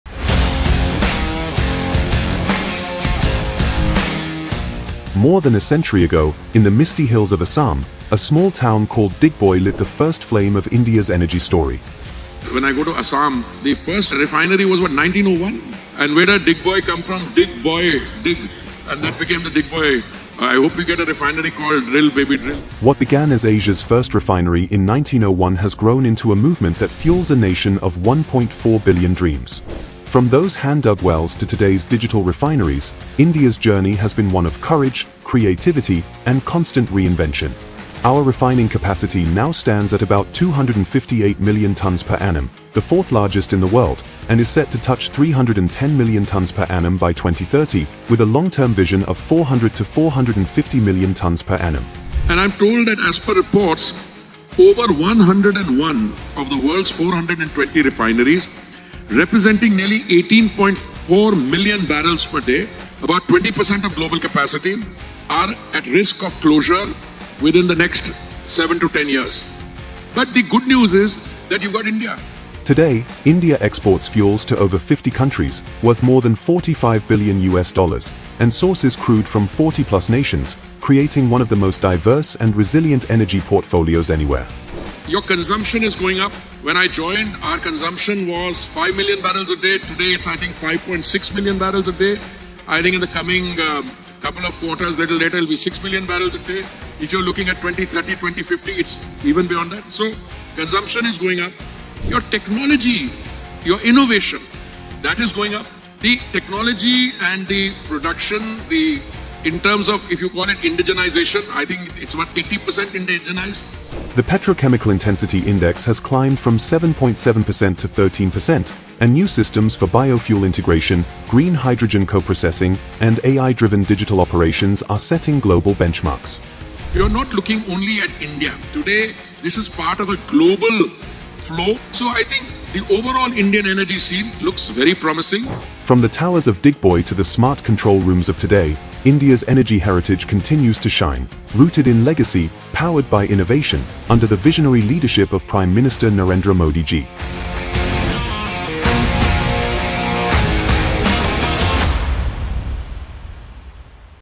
India’s Energy Heritage - From Digboi to Viksit Bharat !
From the first refinery in 1901 to one of the world’s top refining powers today, India’s energy journey blends legacy with innovation.
258 MMTPA refining capacity, 50+ export destinations, 40+ crude sources, and cutting-edge refineries - all shaping the dream of a self-sufficient Bharat under PM Narendra Modi ji.